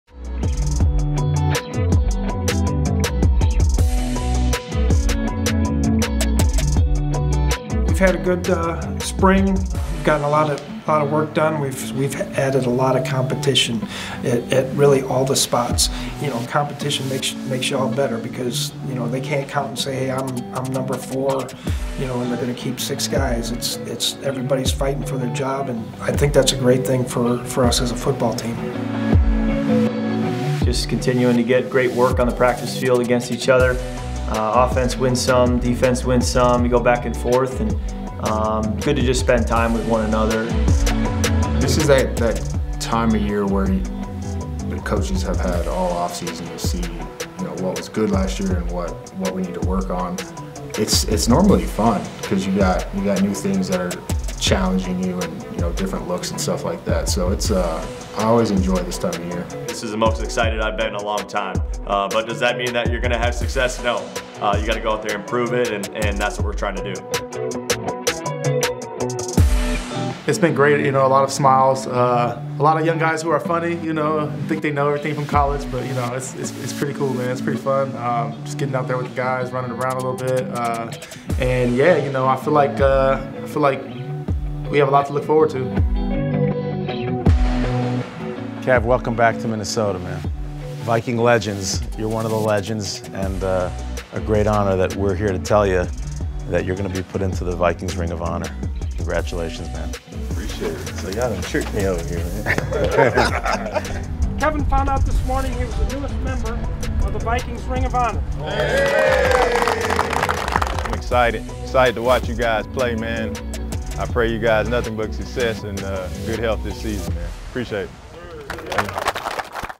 Inside 2021 Minnesota Vikings Minicamp
The Minnesota Vikings wrapped up the 2021 offseason program this week, with mandatory minicamp ending a day early as a reward from Head Coach Mike Zimmer. But it was an action-packed week both on and off the field, including a visit from Kevin Williams, who learned he will enter the team's Ring of Honor later this year.